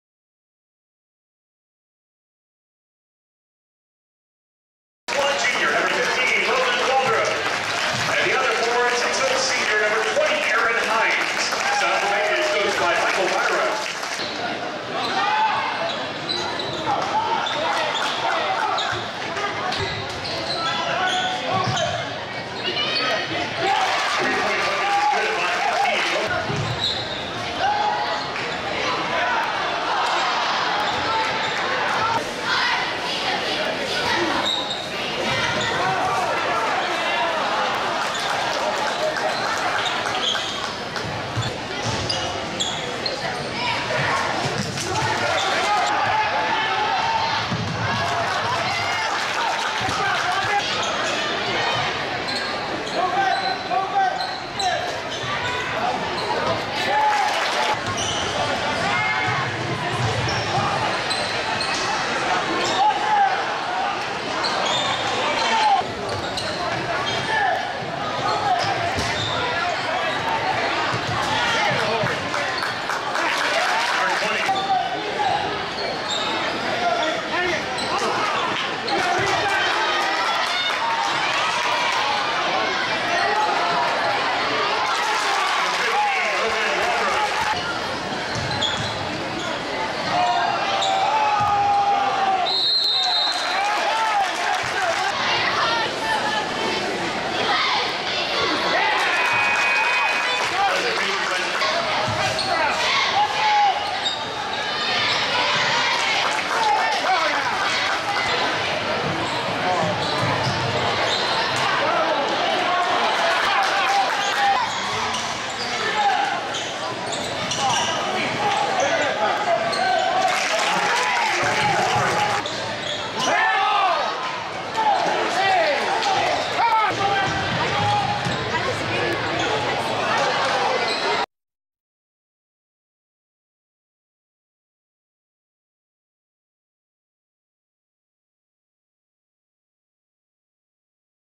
Logan_SV vs WV BB 2013
Varsity basketball @ West Vigo, Dec. 10, 2013